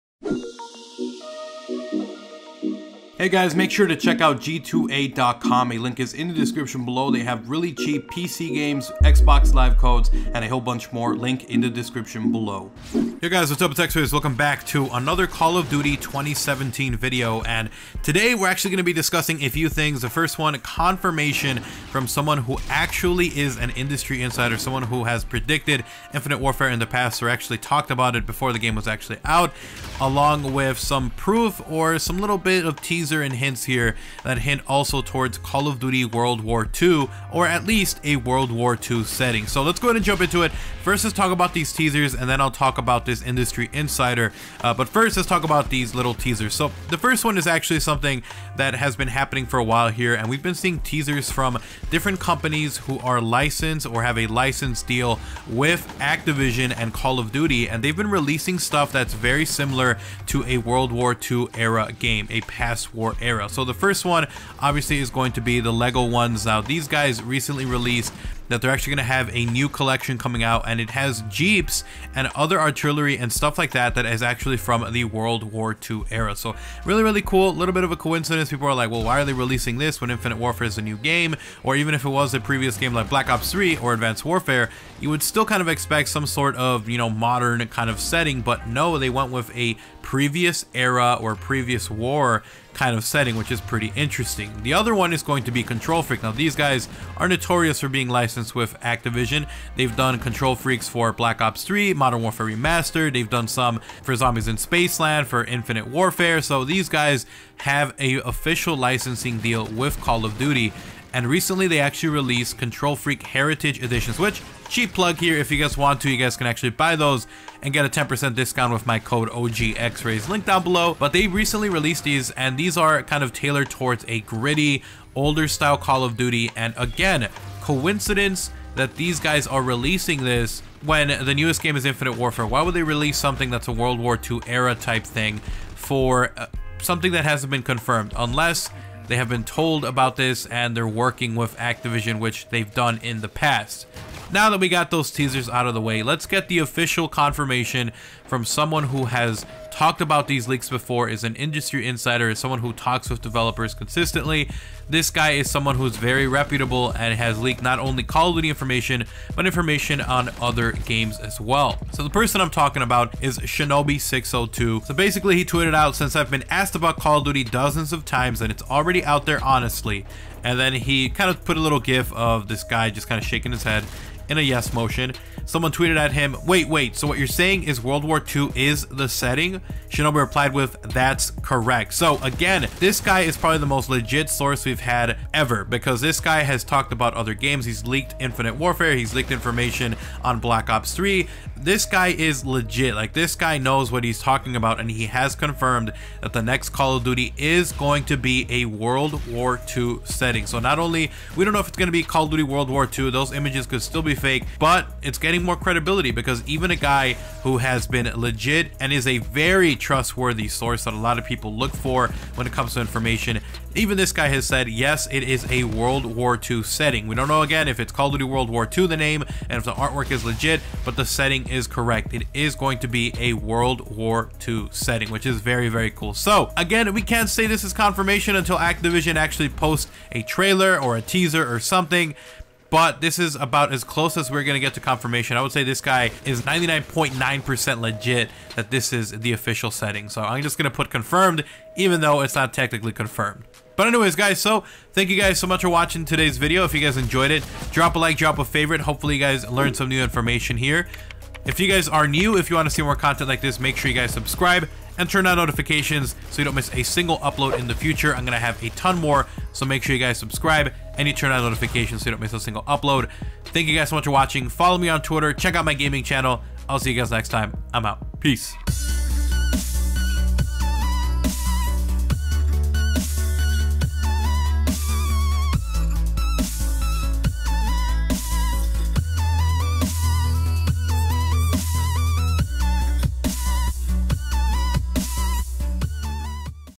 Call of Duty 2017 World War 2 Setting Confirmed By Industry Insider! (Call of Duty: WWII Leak)
It Looks Like Shinobi An Industry Insider Has Confirmed That Call Of Duty 2017 Will In Fact Take Place During WW2! HYPE!
● Enjoyed The Video?

Social Media & Apparel

- OGXRAYZ